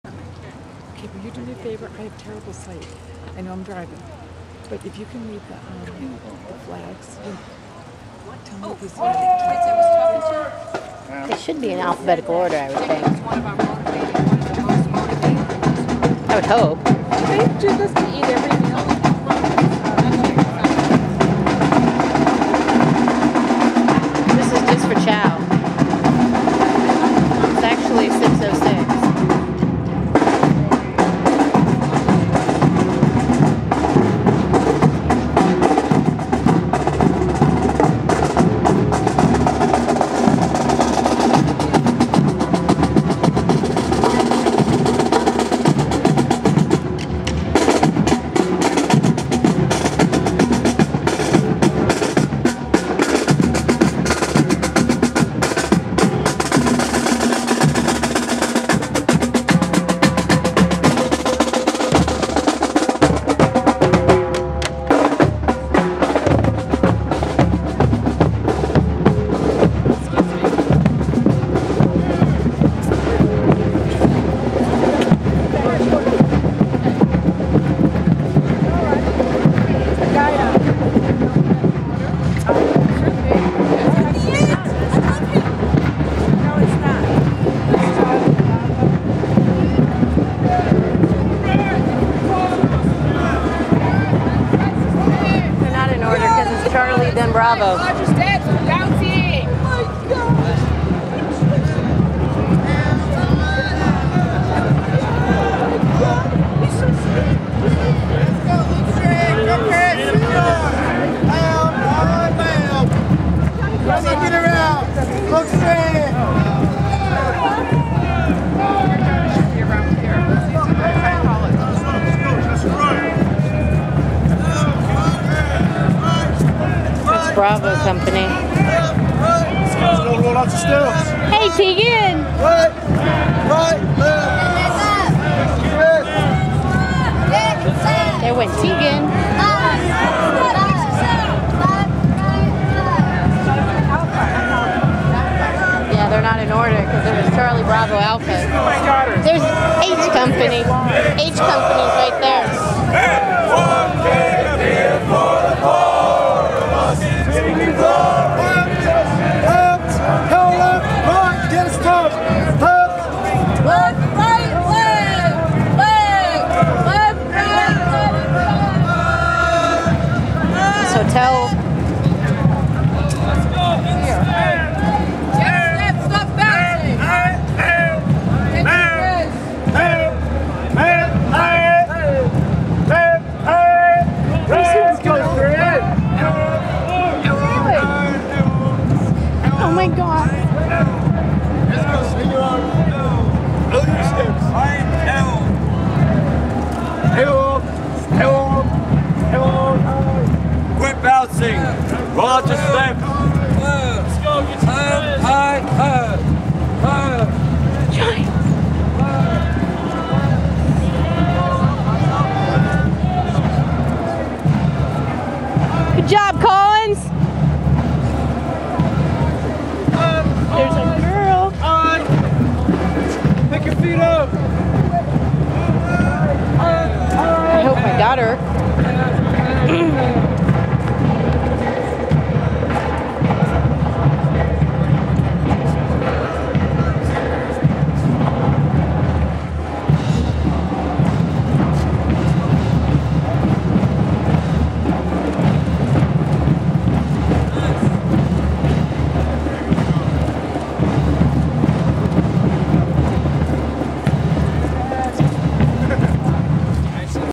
NMMI 8-28-2009 Chow call
New Mexico Military Institute march to chowhall

I guess they do this formation and march for almost all meals. The red tag on their ACUs means they are RATS (newbies). I don't think us parents were suppose to be here to see them yet.. but...well. ahhhhh.. I couldn't resist. :) .. most all the troops are here, and I tried to get them all. until I saw F troop coming and my focus was then on my cadet.

The Cadets here range from high school freshman to college sophomores.  Lots of excited parents were standing around me... you can hear some of them.